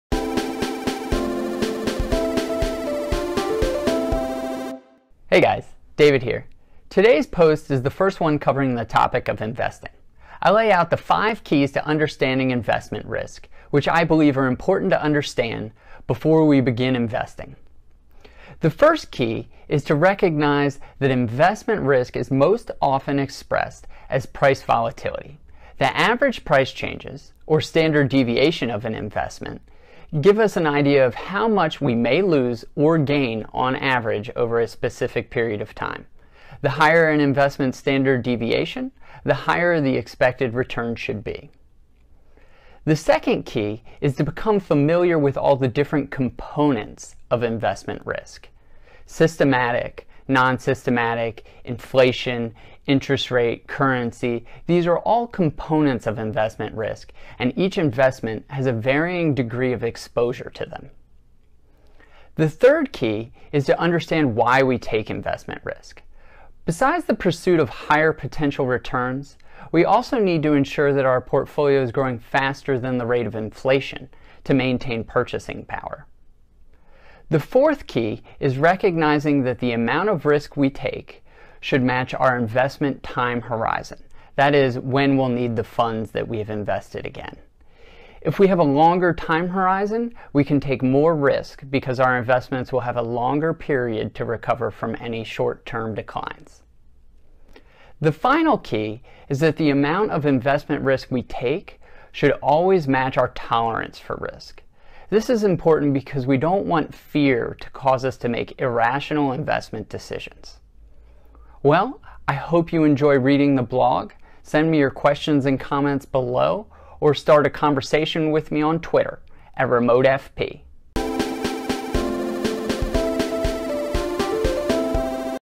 5 Keys to Understanding Investment Risk
To avoid costly investment mistakes we must know the risks involved and select the proper investments for our time horizon and risk tolerance.  Remote Financial Planner lays out the 5 keys to understanding investment risk and avoiding these mistakes.